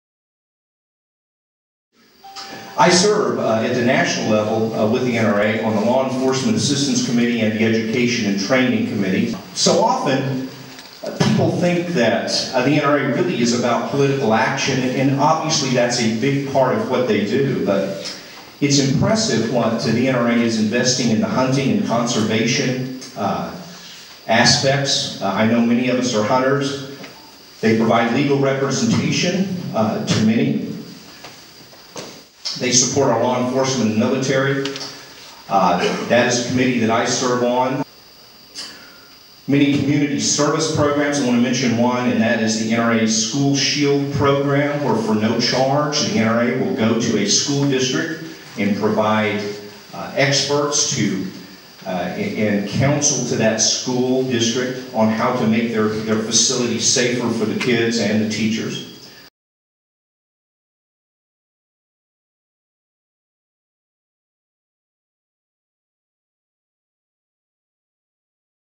OKC Gun Club New Member Orientation
Mark Vaughan, 2019 nominee for the NRA board of directors and Vice President of the OKC Gun Club, shares about some of the NRA's programs at the new member orientation.